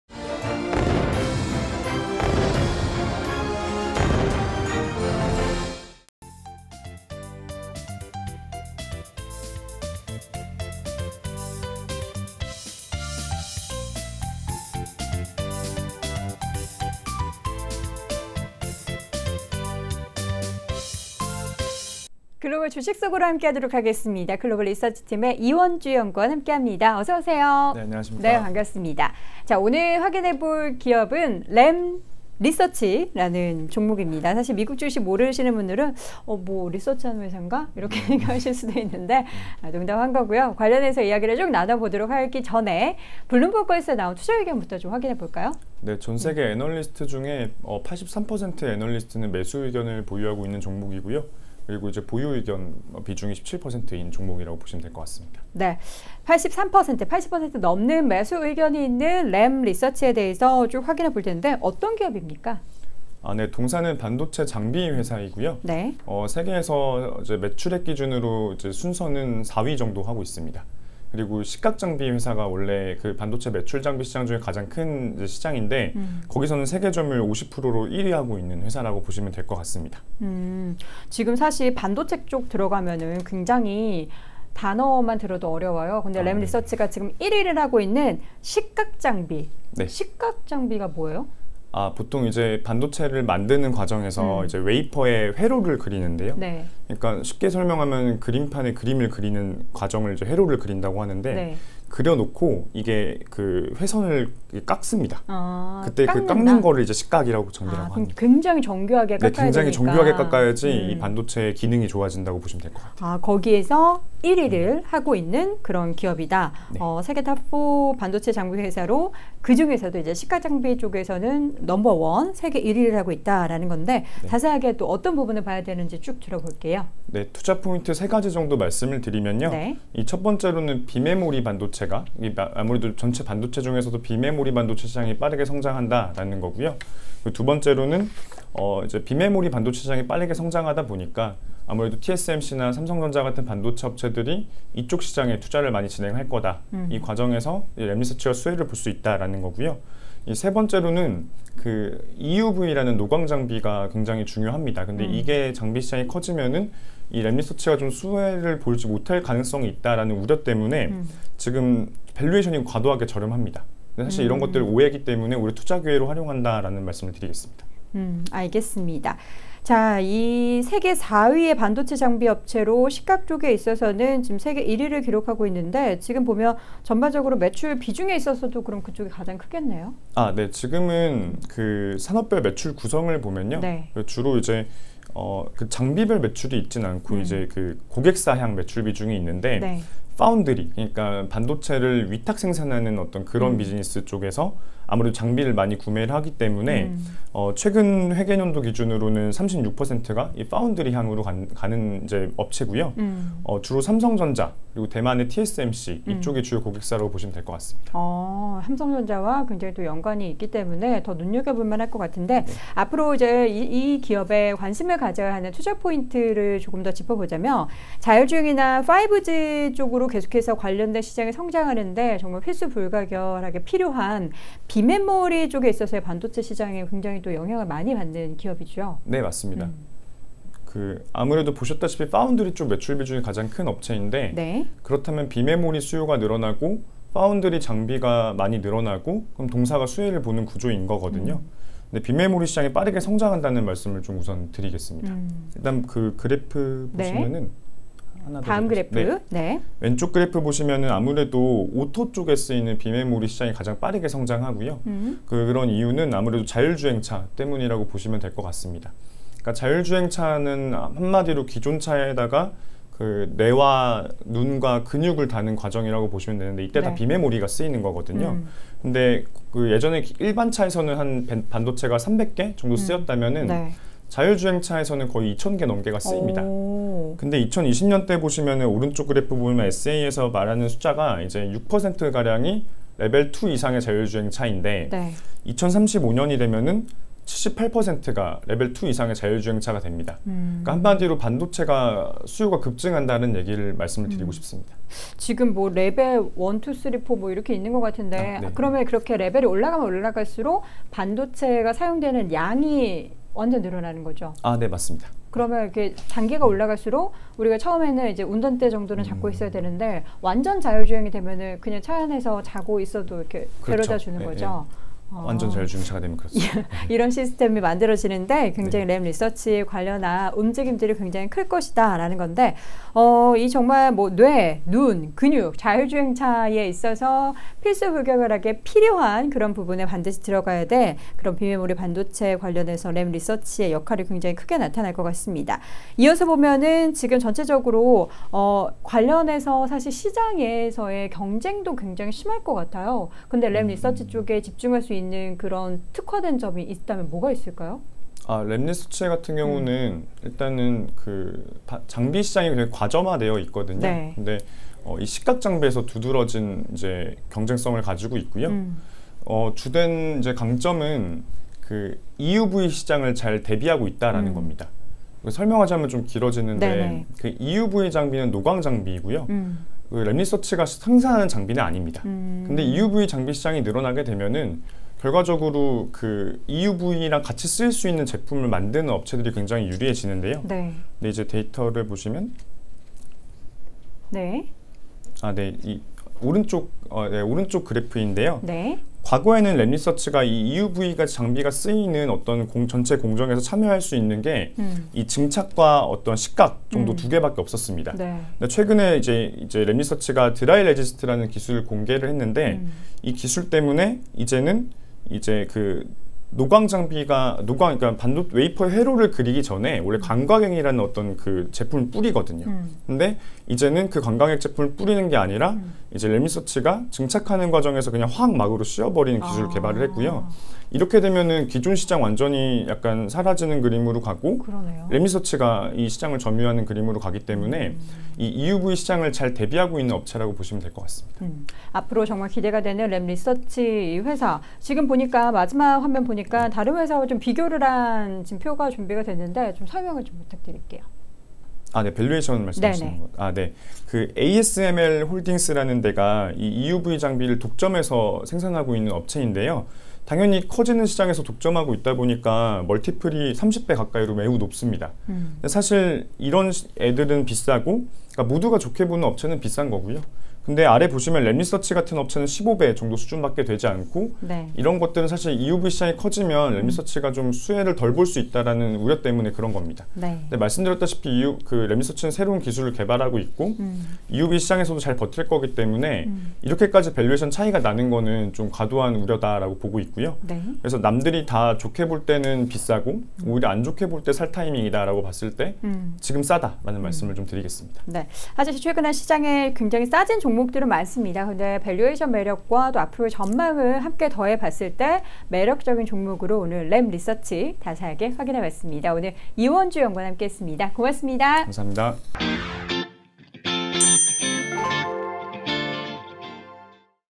​[해외주식투자] 글로벌 주식 속으로/ TSMC vs 삼성전자 경쟁이 수혜인 성장주 ​(20.04.10)​
램리서치
시계 Top4 반도체 장비회사
반도체 장비 시장이 줄어도 실적은 견조함

​2019년반도체 장비 매출액 기준, 세계점유율 14.2%로세계 4위반도체 장비 회사
​가장큰 시장인 식각/세정시장에서는 점유율 45~50%로1위
​삼성전자, TSMC 등파운드리향매출 비중이 36%(2Q20FY기준)로가장 큼
​기존 고객사들로부터AS,업그레이드등의 매출이 지속적으로 발생하는 것이 강점​

​비메모리반도체 중심으로 성장하는 반도체 시장(비메모리시장 2018~2022 CAGR +4.8%)
​TSMC, 삼성전자 경쟁적으로 EUV 장비 도입할 예정
​이 과정에서 동사 식각, 증착 장비 매출도 증가
​EUV로인한 동사 매출 성장 감소 우려로 저렴해진 밸류에이션​
​다만 이는 기우,​ 투자 기회​
​여도은 키움증권 채널K 앵커, 이원주 연구원 글로벌리서치팀
​​ 「구독」과 「좋아요」 눌러주세요~